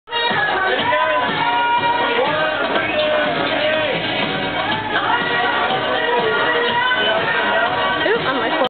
American Idol Top 24 Party - STK in West Hollywood, CA 2/18/10
I meant to take a quick pic, but accidentally took a lil' video instead.